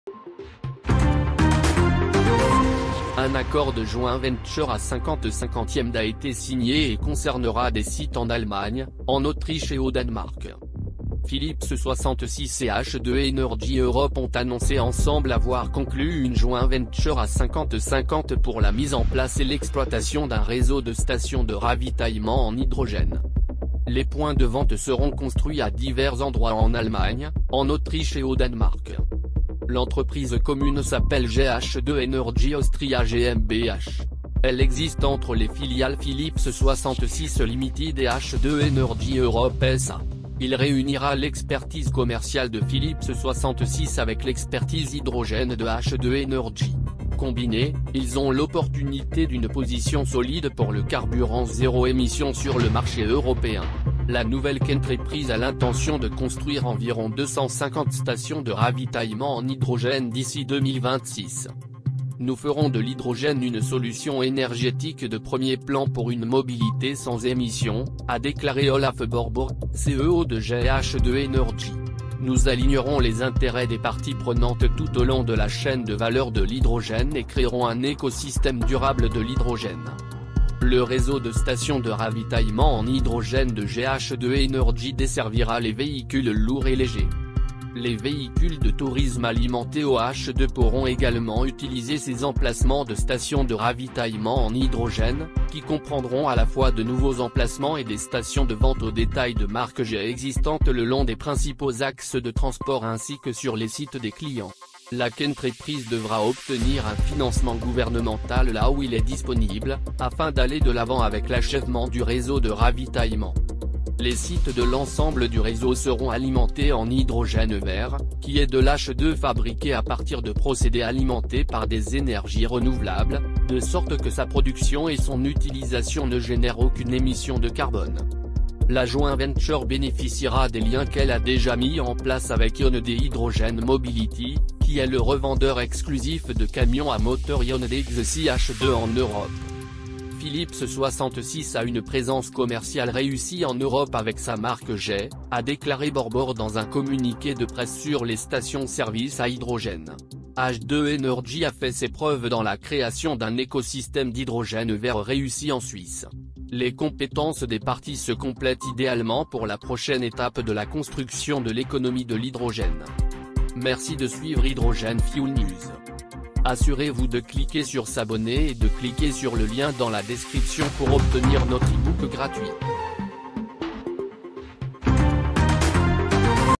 French Version - Phillips 66 and H2 Energy Europe collaborate on hydrogen refueling stations
Phillips 66 and H2 Energy Europe have together announced that they have entered into a 50-50 joint venture for the establishment and operation of a network of hydrogen refueling stations - For full article